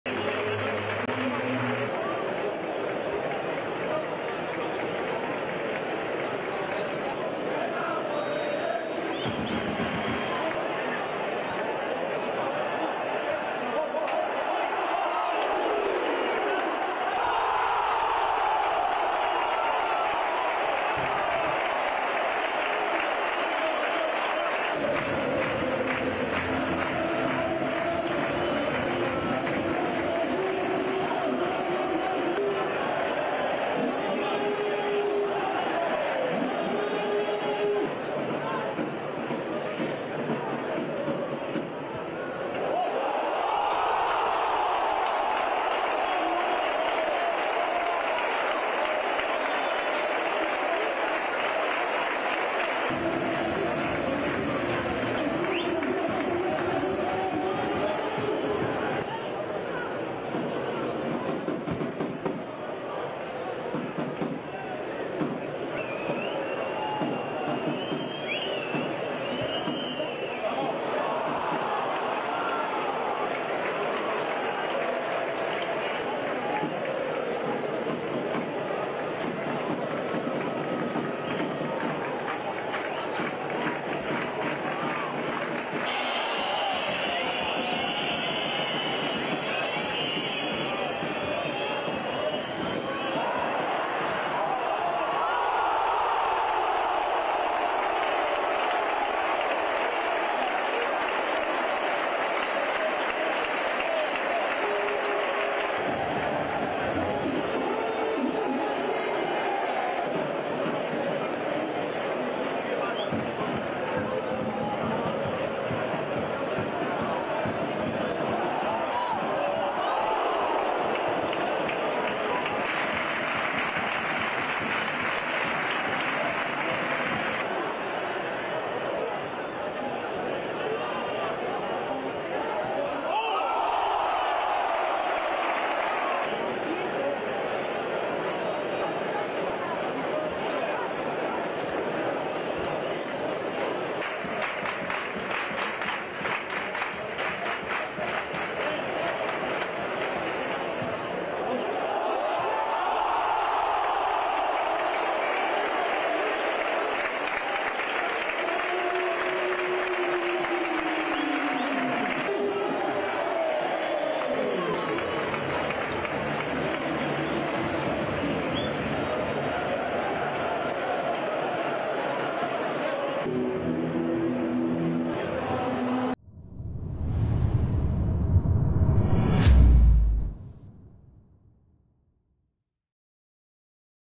Resumen PrimeraFederación | CD Castellón 5-1 AD Mérida | Jornada 6, Grupo 2
⚽ Manu Sánchez (0-1, 6')
⚽ Manu Sánchez (0-2, 18')
⚽ José Elo (2-1, 31')
⚽ Jesús de Miguel (3-1, 38')
⚽ M. Bonaque p.p (4-1, 46')
⚽Manu Sánchez (5-1, 62')